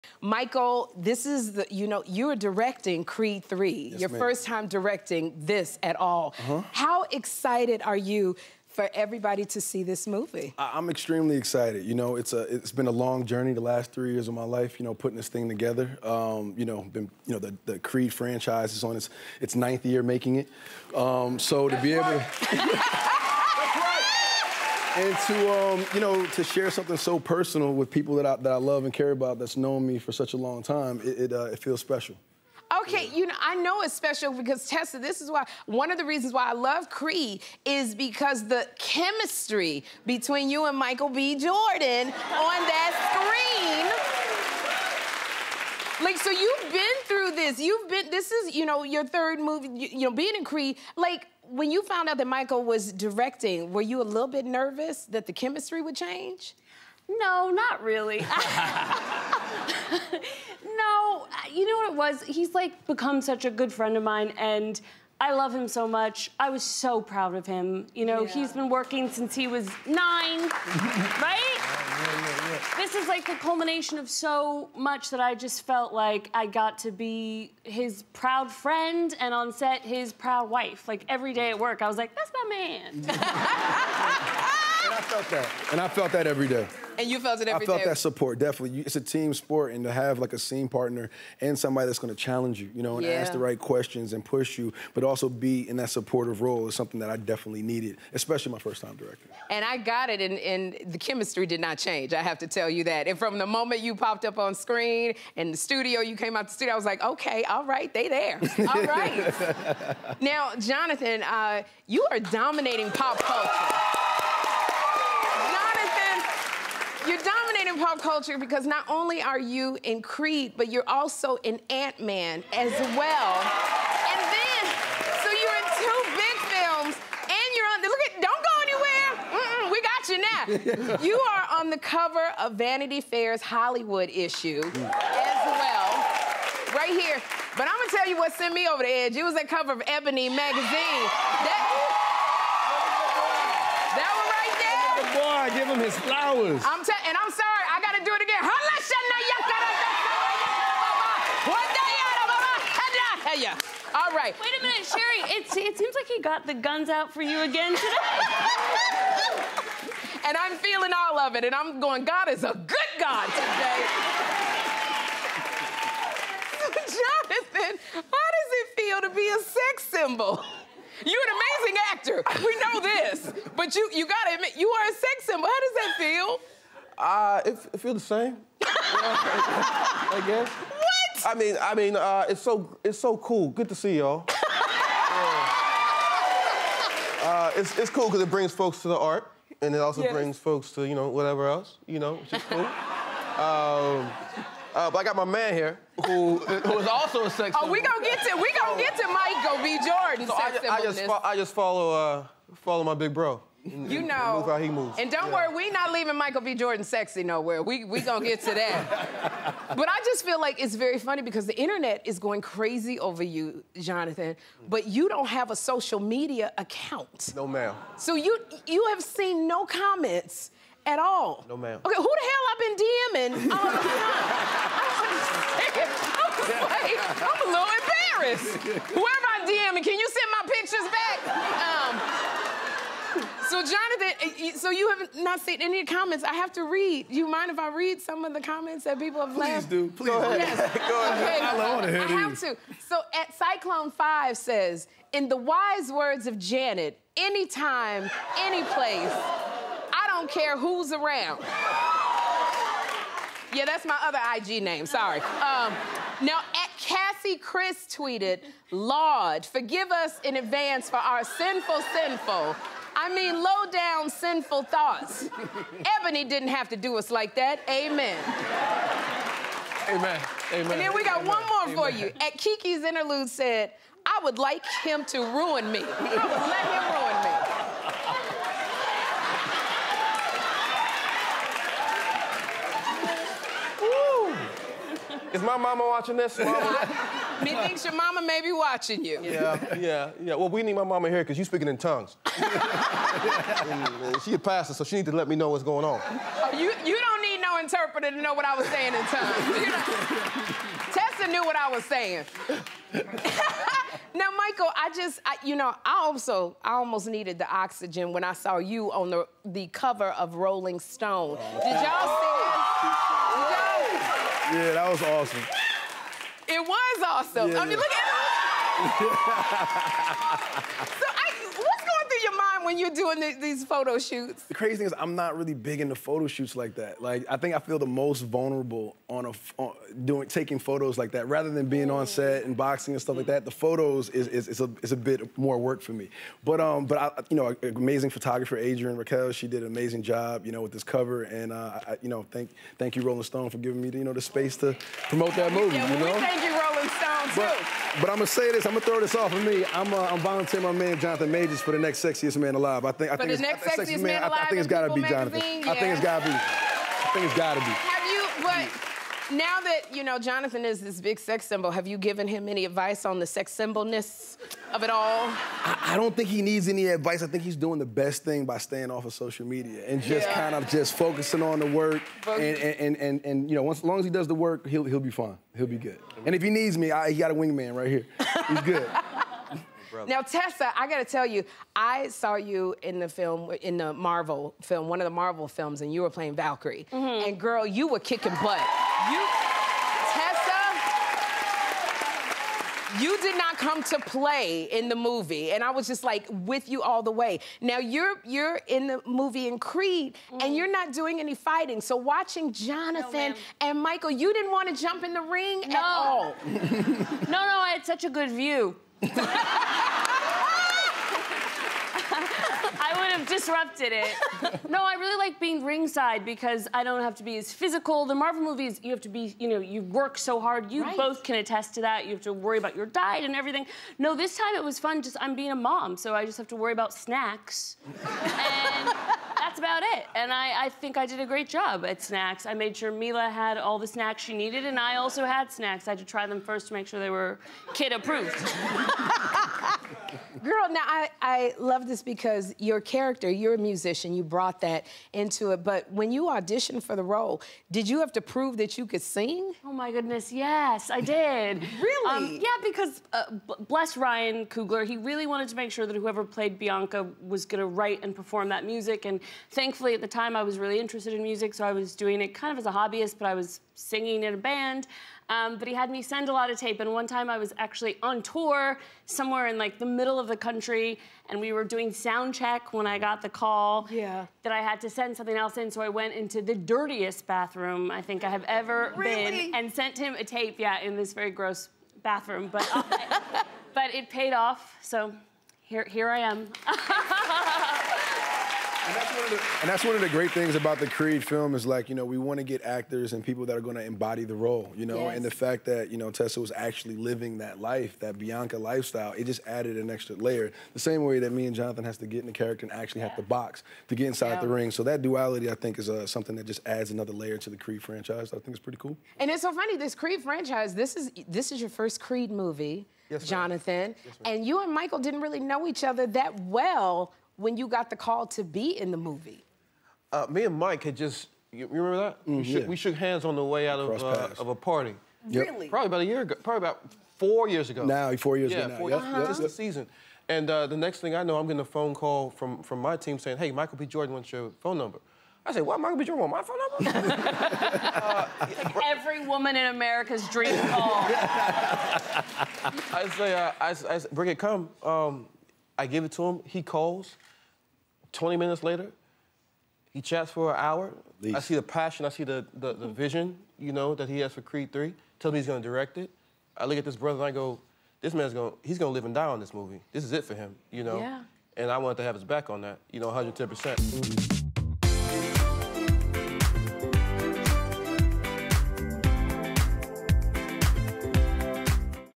Michael B. Jordan, Tessa Thompson & Jonathan Majors
TOTAL KNOCKOUTS! 🥊🥊🥊 Sherri catches up with Michael B. Jordan, Tessa Thompson and Jonathan Majors about “Creed III”, Marvel superheroes and of course *those Ebony magazine pics!